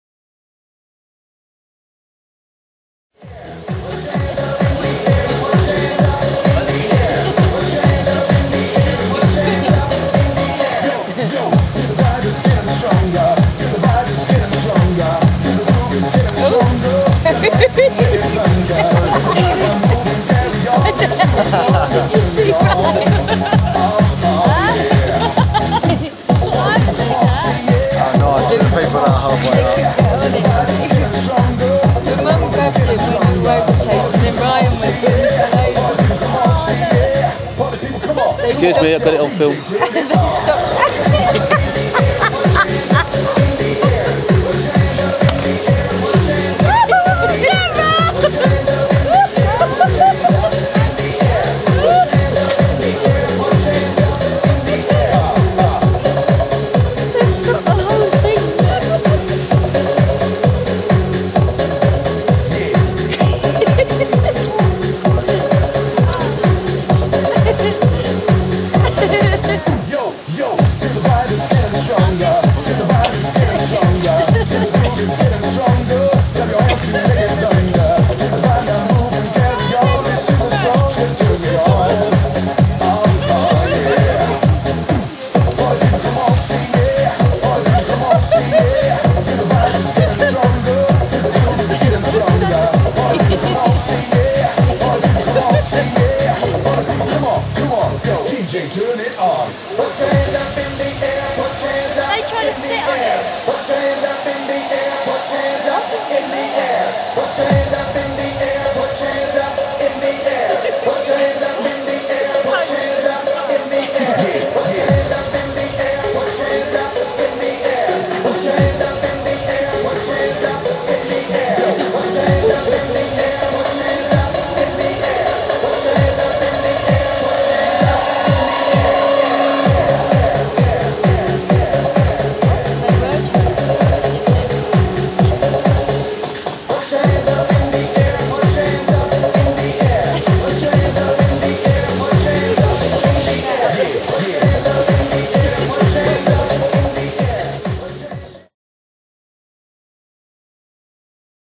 First time on a T bar
The first few attempts to grab a T Bar by my wife and a friend!!!